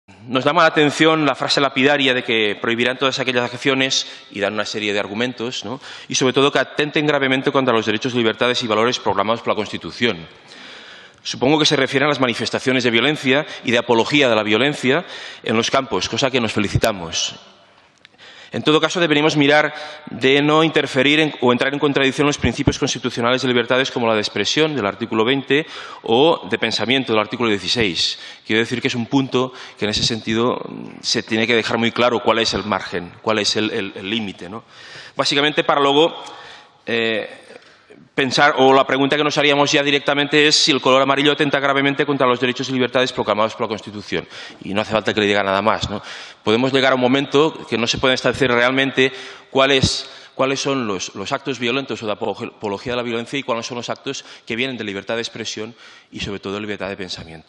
Xavier Eritja al ministre Guirao: ' el color groc atempta greument contra la Constitució?'
El diputat d'Esquerra Republicana formula aquesta pregunta al titular de la cartera d'Esports i Cultura del Govern Espanyol quan aquest informava en una Comissió al Congrés sobre l’avantprojecte de Ley del Deporte